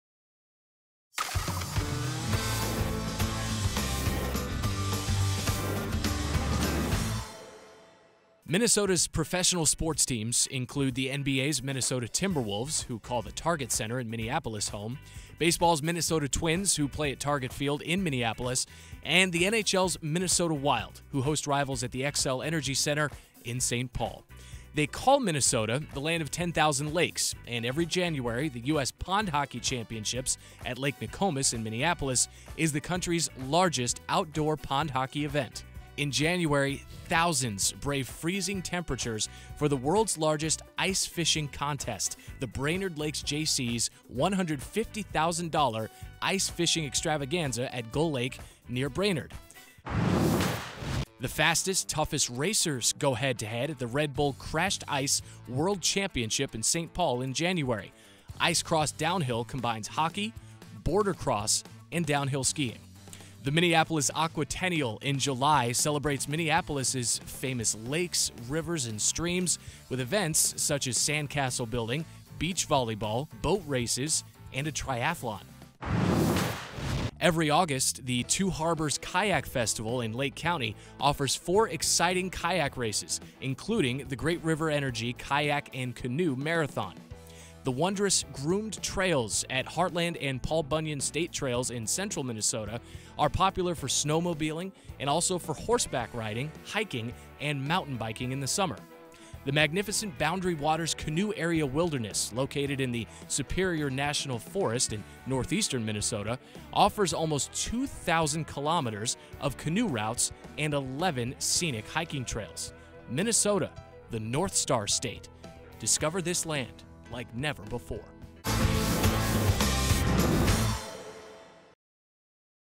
Minnesota Sports and Recreation
Minnesota: Known for its cool temperatures, thick forests and abundant lakes, Minnesota has a lot to offer any visitor. Catch one of the professional sports teams that call Minnesota home like the Timberwolves, the Twins and the Wild. Take advantage of the colder months by watching the US Pond Hockey Championships, the Crashed Ice World Championship, or kick back and try ice fishing!